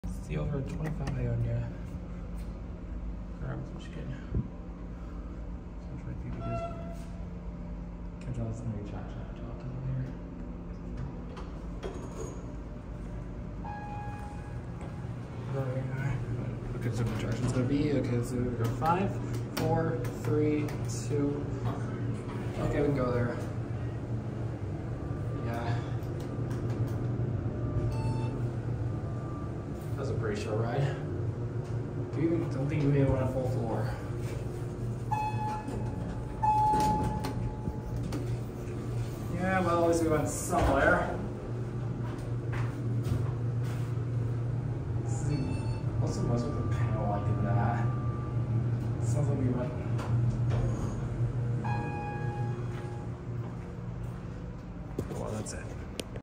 Mostly Locked MontgomeryKone traction Elevator @ 25 Ionia Ave Grand Rapids, MI
The panel suggests this is a mod, but I can’t tell what it would have been originally.